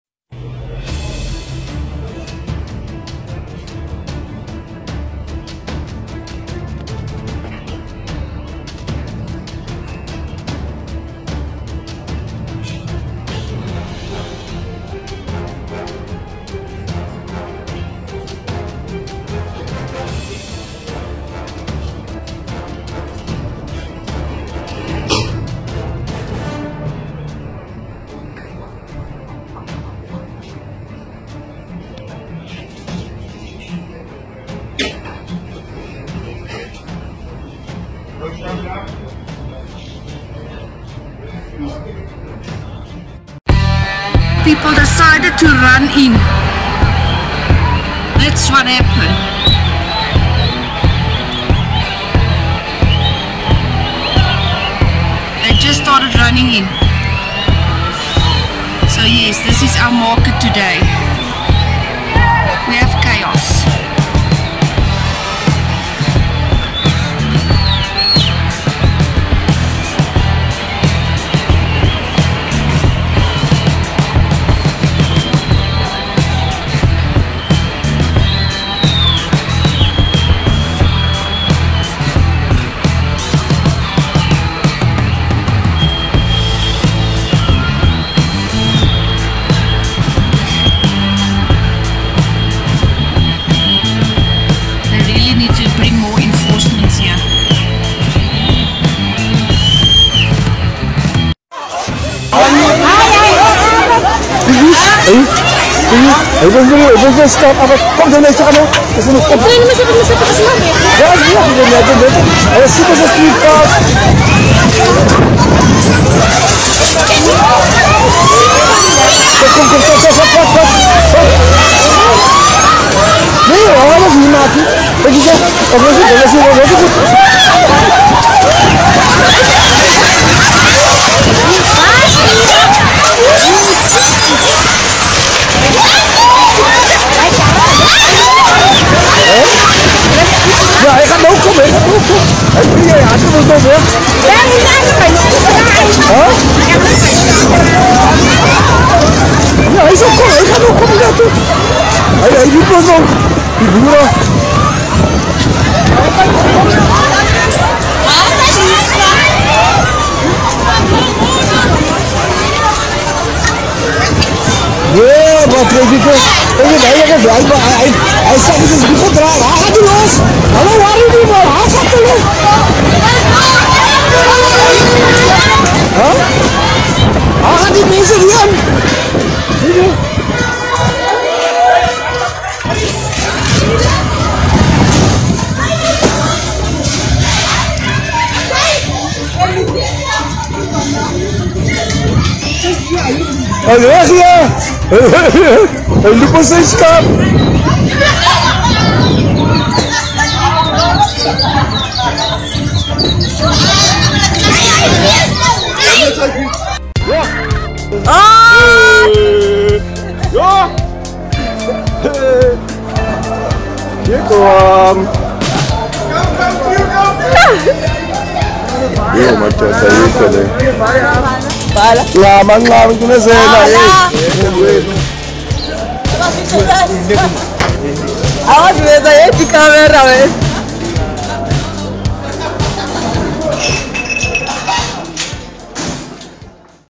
COVID-19: LOCKDOWN IN SOUTH AFRICA HAS COLLAPSED
Guys are out in the street because of hunger